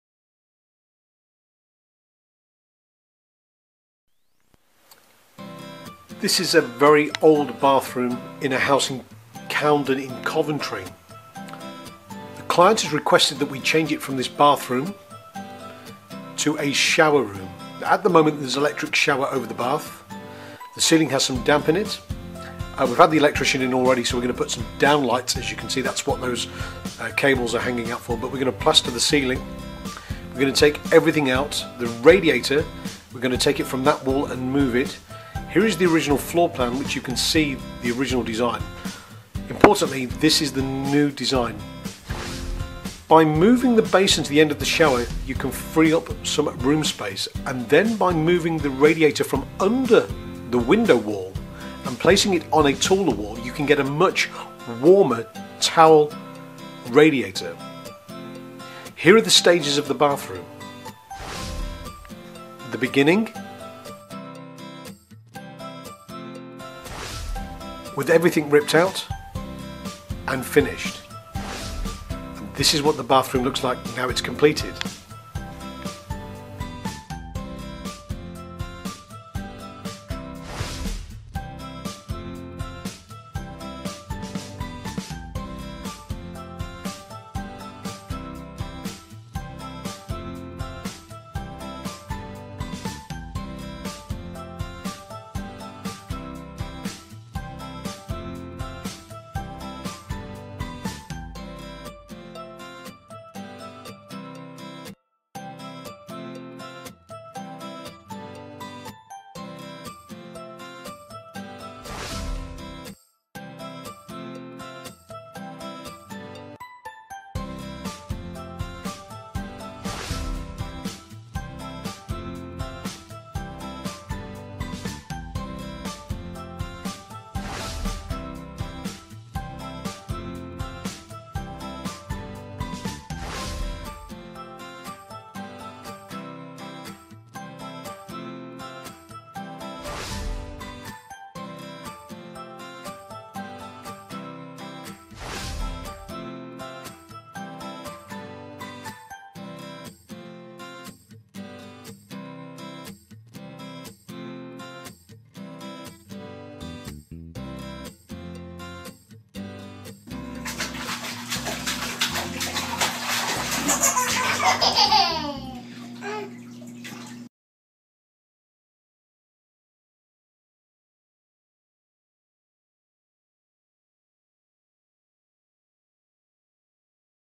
Bath to Shower Remodel Coventry
This is a bathroom in a house in Coundon Coventry that we converted to a shower room. The video shows the bathroom before and the shower room after. Images include the floor plans. Coventry Bathrooms redesign bathrooms to shower room for clients around Coventry